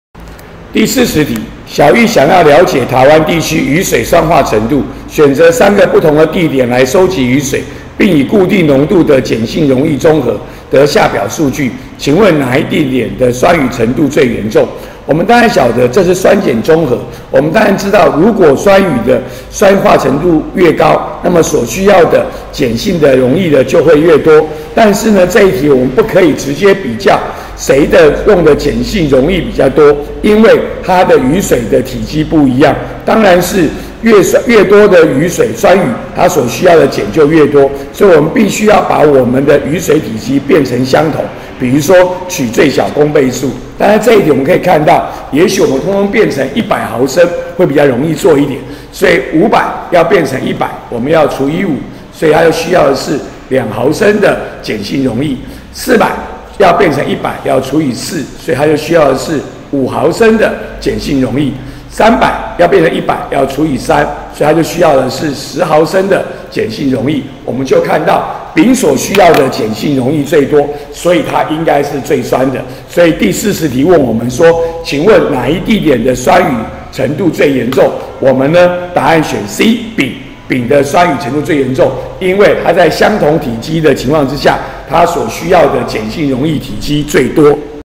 103學年下8年級理化二段第40題
給不同雨水體積，及中和所需鹼性溶液的量，問哪個雨水最酸。